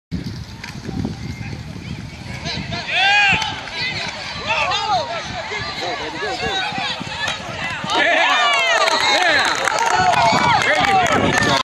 Motell Parker TD run Dickinson Gators
Montell Parker hard td run against La Porte A team